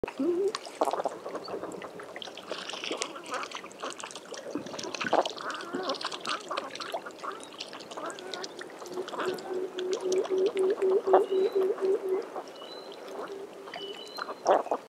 Graceful Spectacled Eider: A Glimpse of Arctic Elegance 🦆
Immerse yourself in the world of Arctic elegance with the beautiful Spectacled Eider